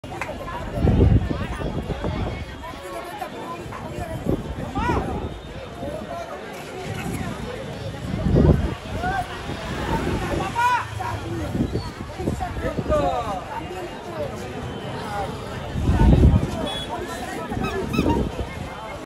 Puri Jagannath Temple - a moment to remember
A few seconds standing in the auspicious environment of the temple premises.